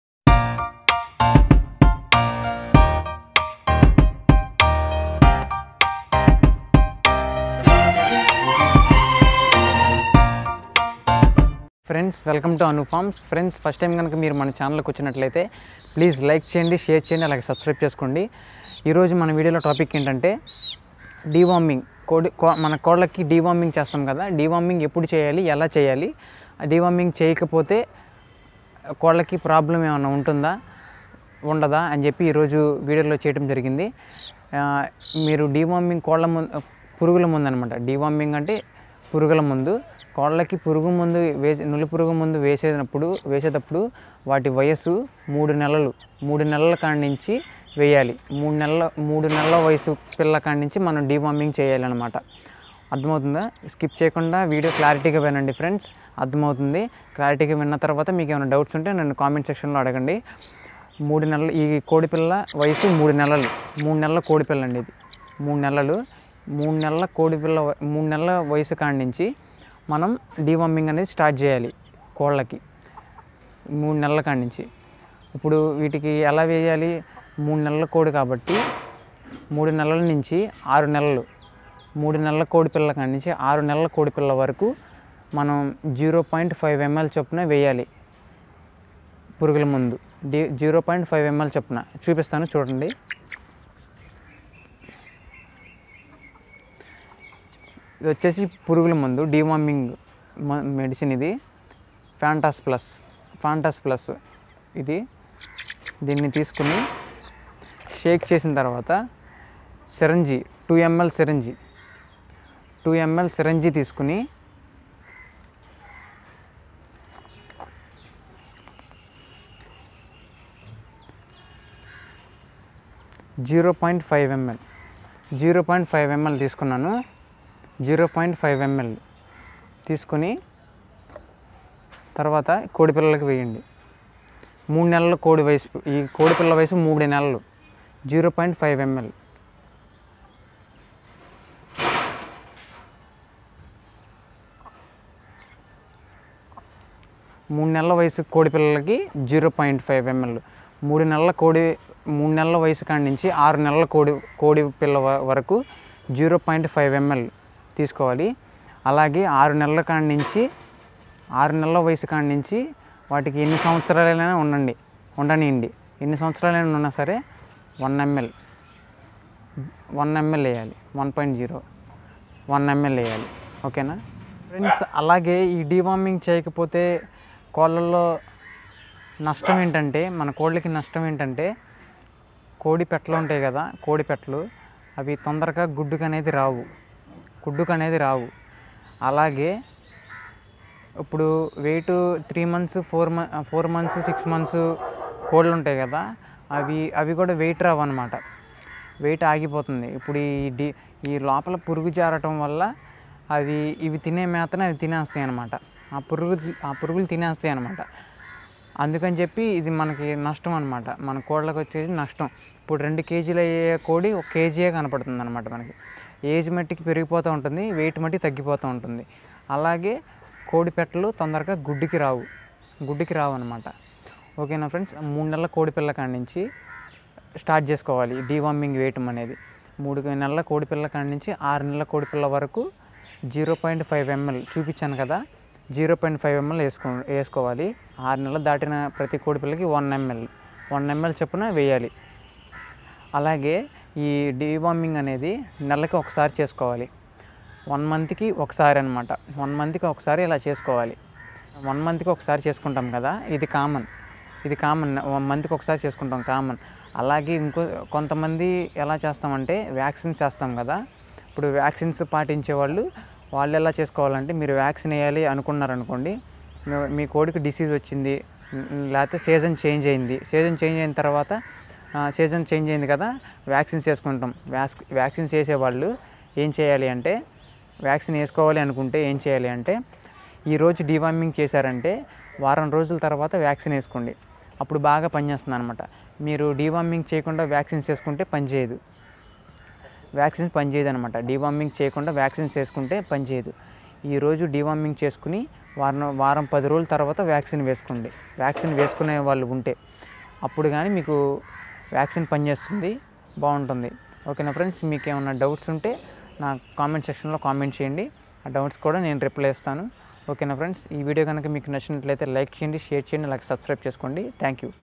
కోళ్ళకు పురుగుల మందు ఎప్పుడు చేయాలి?
ఎలా చేయాలి? చేయకపోతే ఏమవుతుంది?